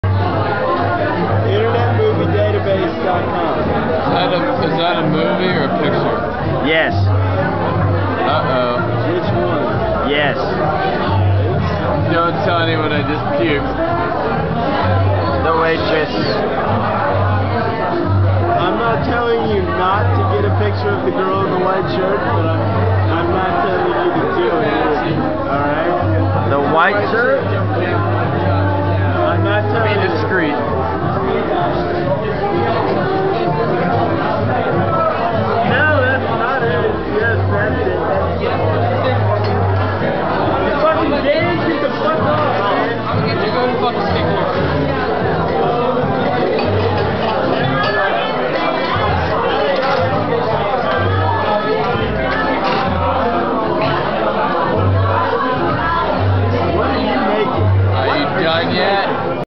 Don't Tell Anyone that Stew Just Puked.
Stew has projectile vomitting after a shot of Absynth. Eesha wants a video of hot, Portuguese girl. Dmitri is enjoying Lisbon at it's best.